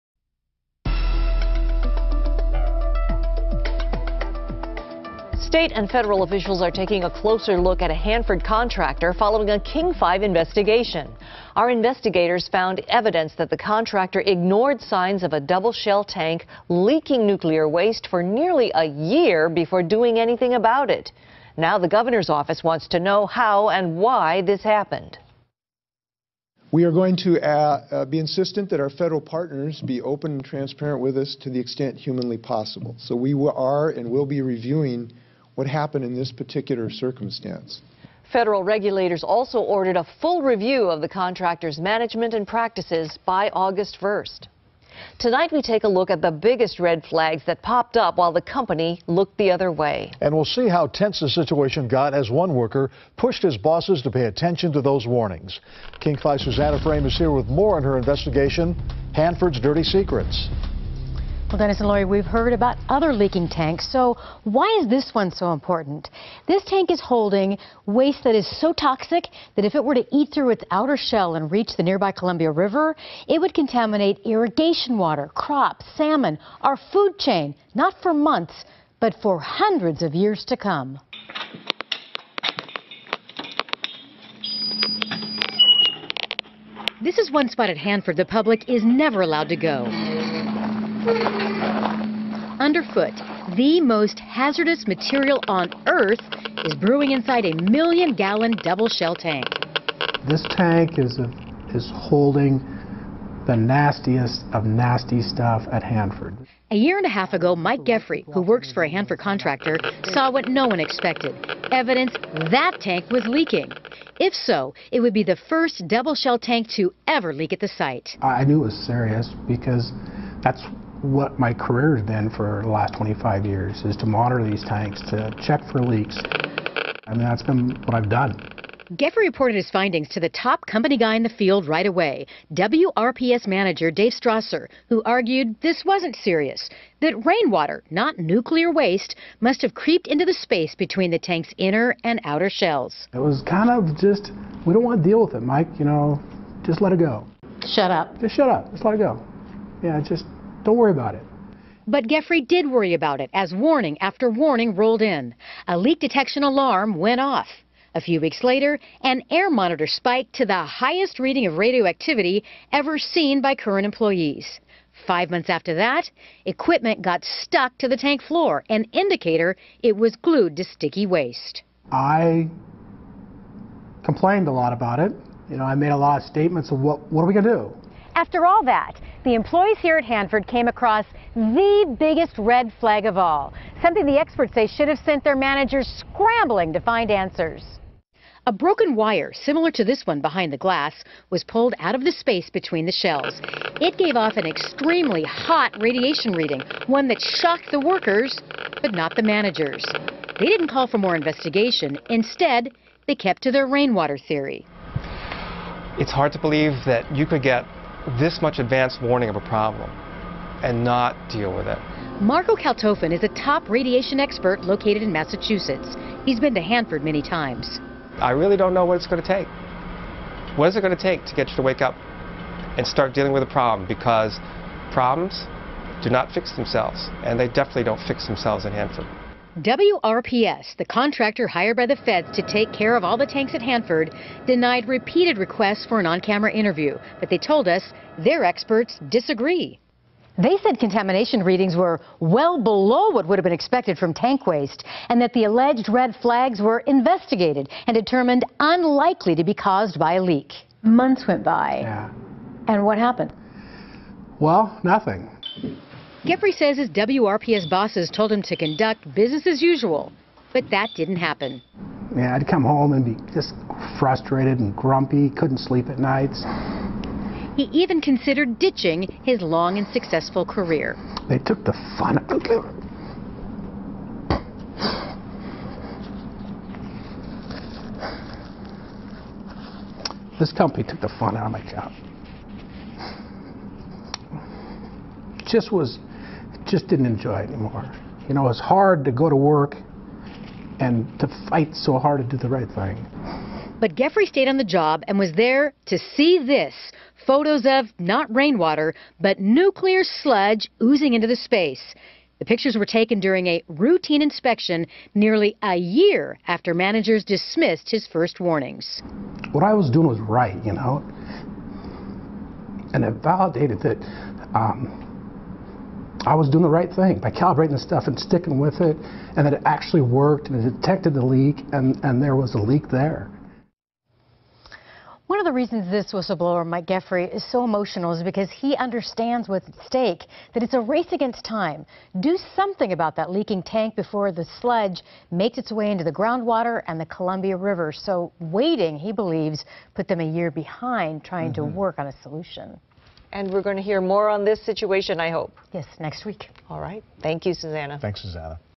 High level waste leak ignored
Special investigative report by Seattle's King5 TV looks at how Hanford workers struggled to report a high level waste leak while others struggled to ignore it.